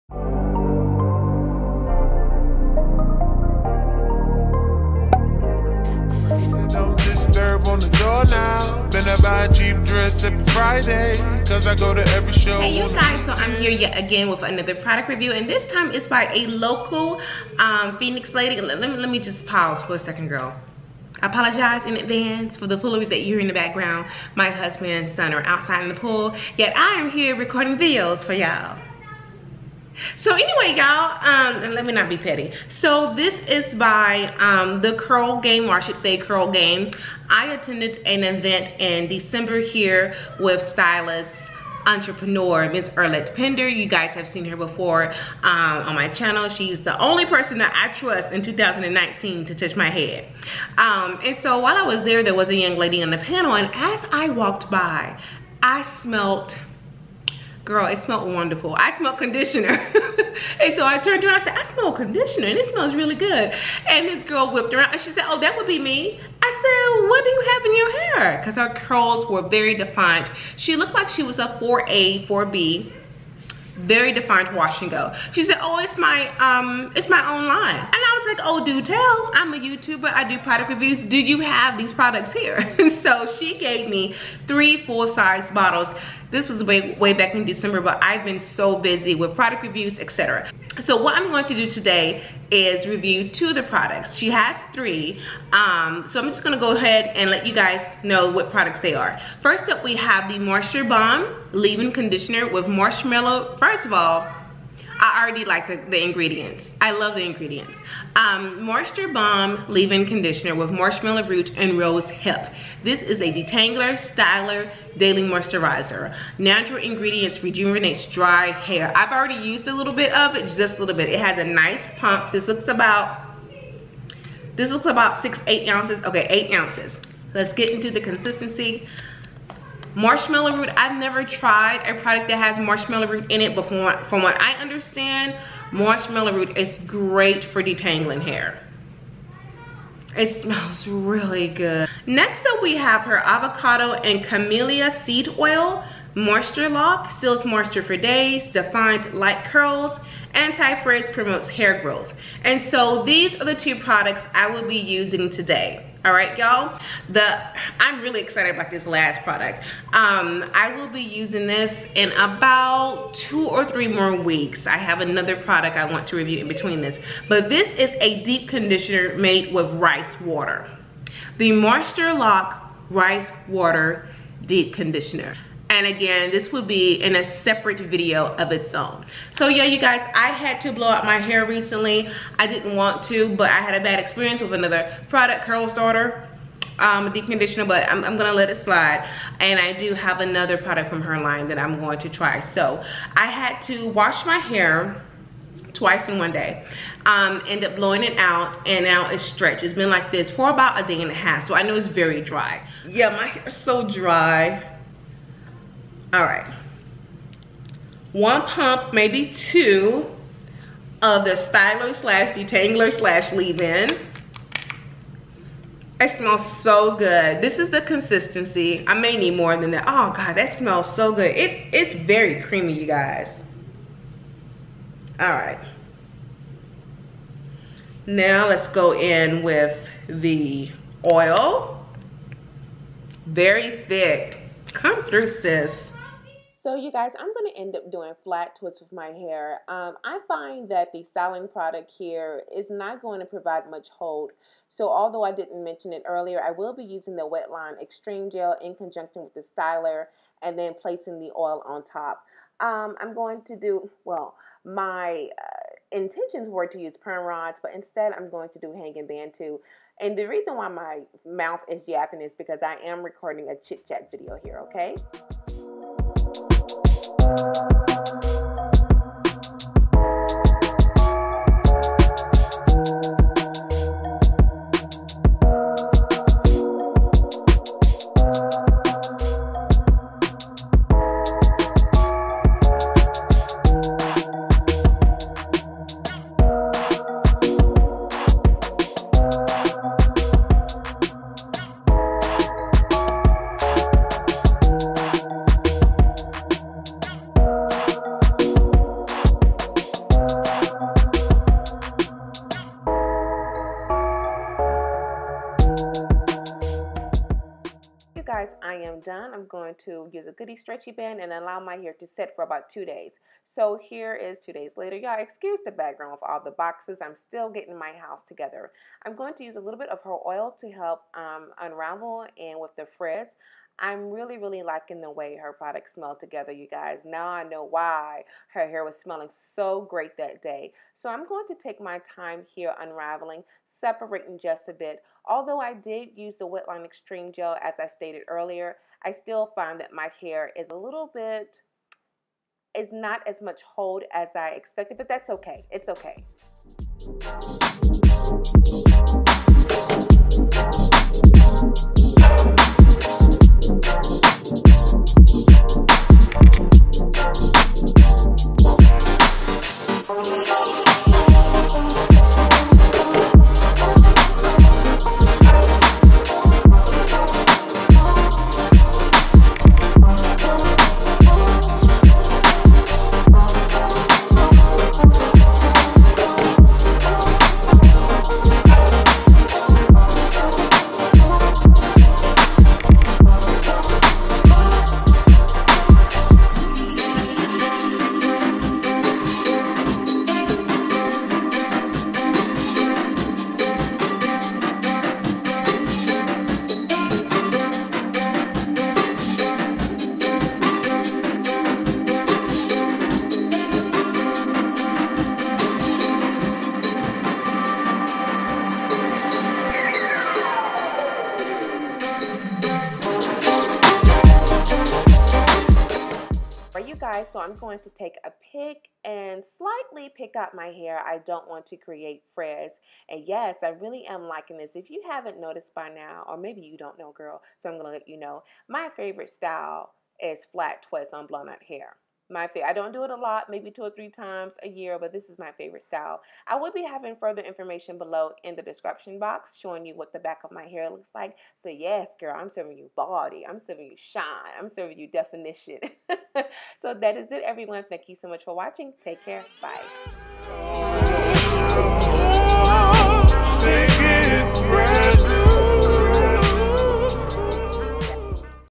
Flat Twists Featuring Curl Game (Black Owned Hair Products)
Let's support small businesses yall!! Today's feature is Curl Game. She offers several different products and is currently running a sale.  The products have no sulfates, parabens, or mineral oils.

Curl Game Moisture Bomb Leave In Conditioner
Curl Game Moisture Lock

EQUIPMENT USED
🔸 LAPTOP: MAC